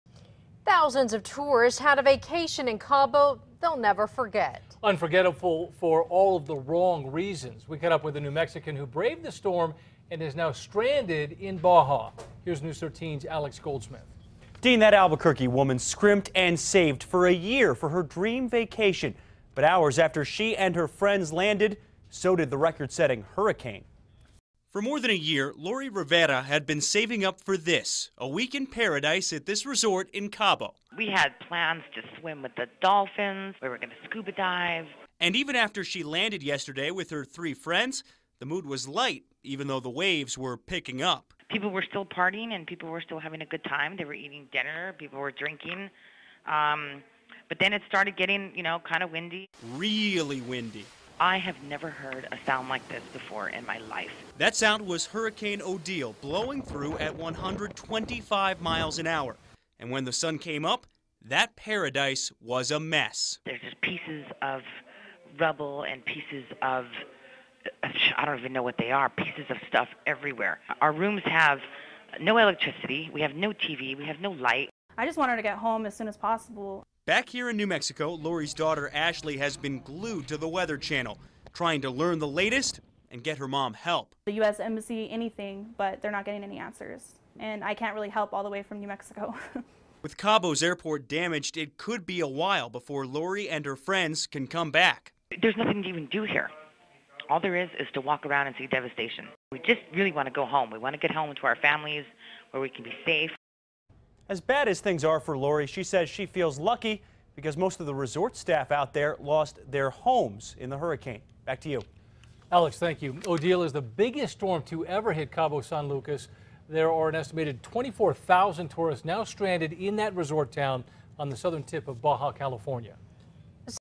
NM woman, friends stranded by Hurricane Odile
A New Mexico woman who saved for more than a year to take a trip to Cabo San Lucas only enjoyed that paradise for a few hours before Hurricane Odile came rolling through.